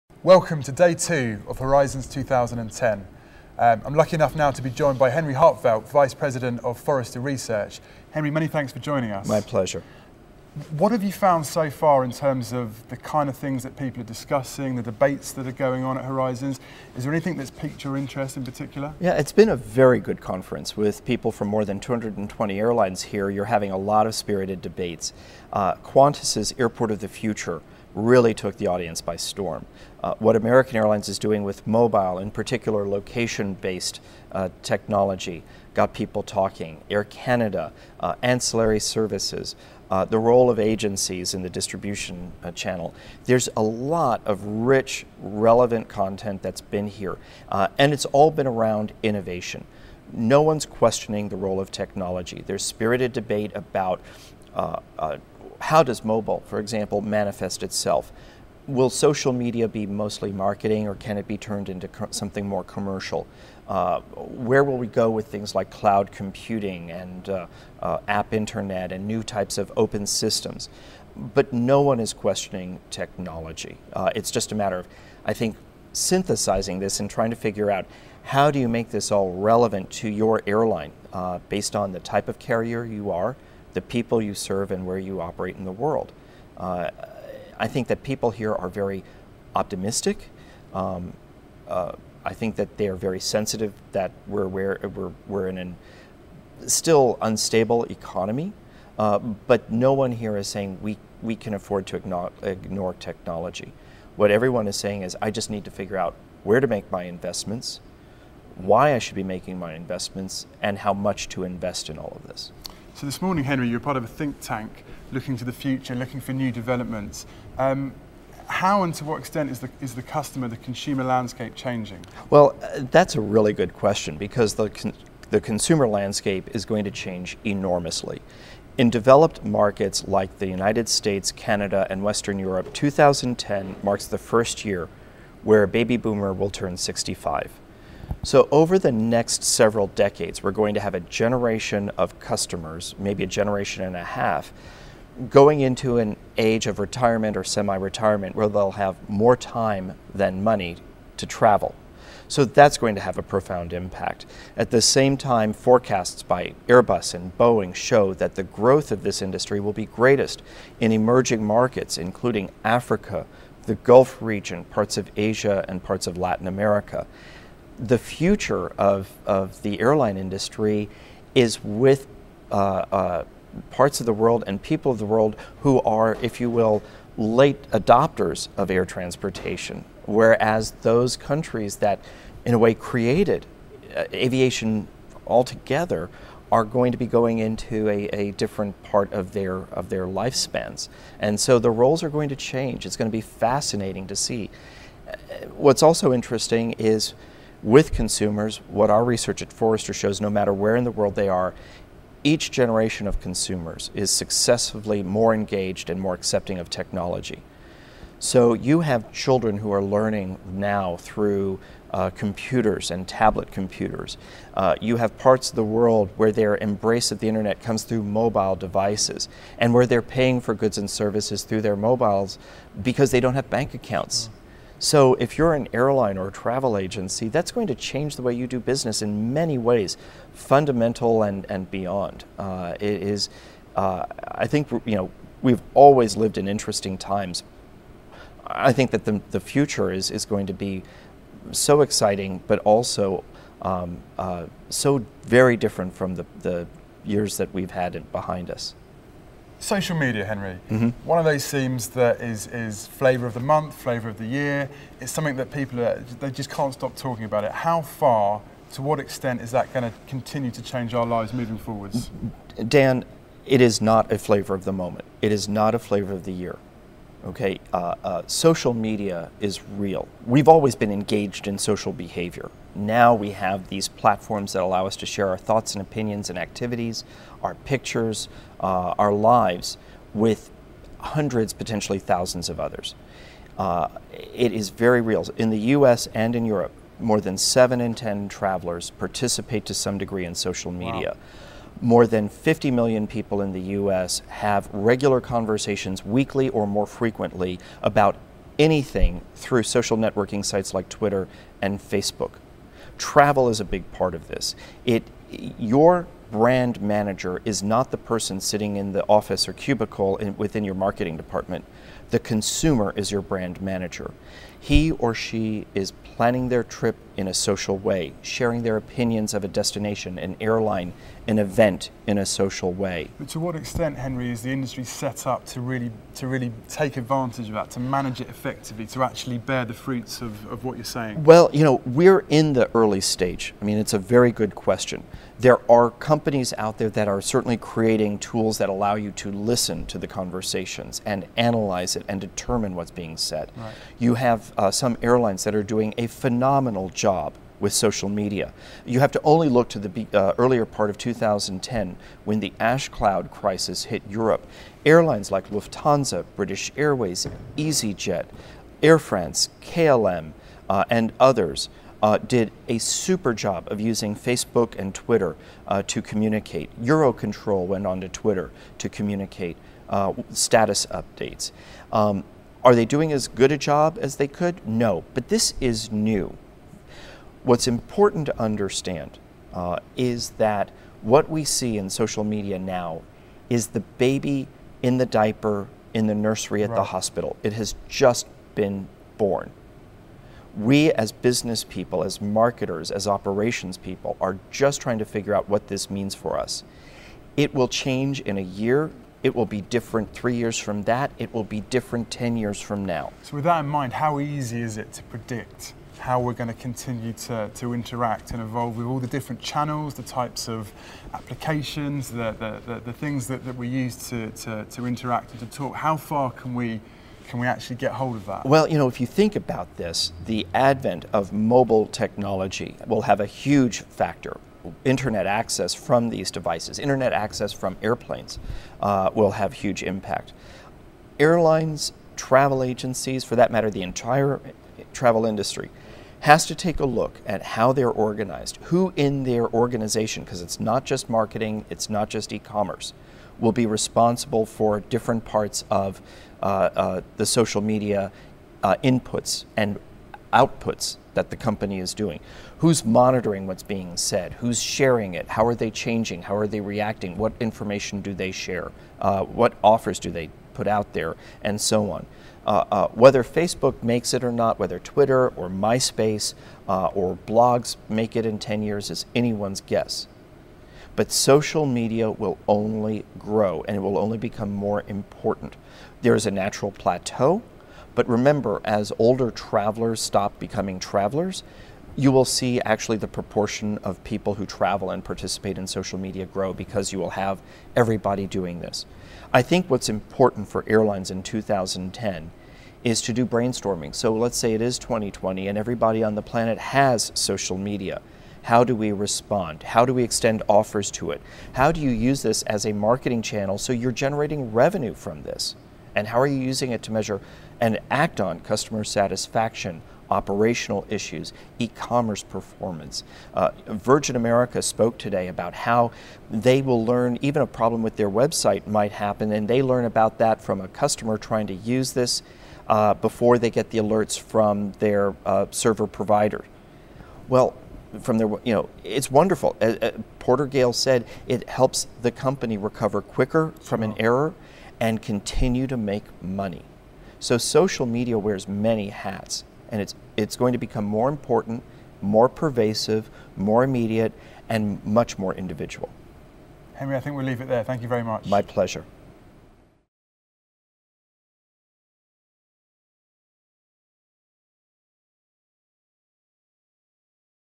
Henry Harteveldt on Horizons 2010 Day 2 - Brighter, Bolder, Better Innovation
Forrester Research VP Henry Harteveldt and Amadeus' Daniel Batchelor discuss how social media and new technology such as Facebook, Twitter, cloud computing, mobile devices, and mobile payments are affecting airlines, emerging markets, changing consumers, and the future travel industry. Get the latest post about cloud computing